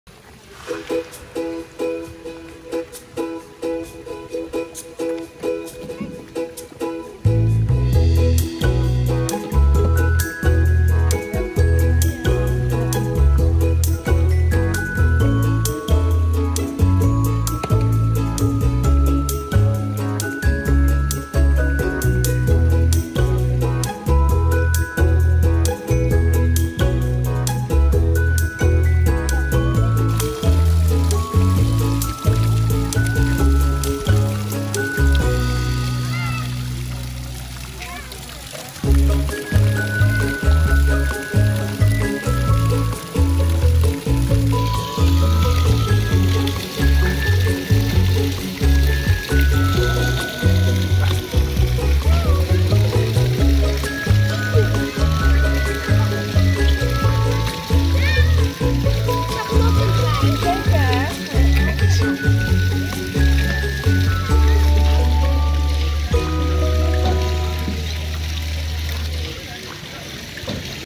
Cute Pomeranian Playing On a Dike! 🐶🧡
Cute Pomeranian playing on a dike. It was a play area meant for children. But Yuki was so curious that he wanted to play there aswell.

Yuki is a Pomeranian dog. He is born and living in the Netherlands. Yuki is a funny and cute Pomeranian puppy. Yuki's color is cream sable.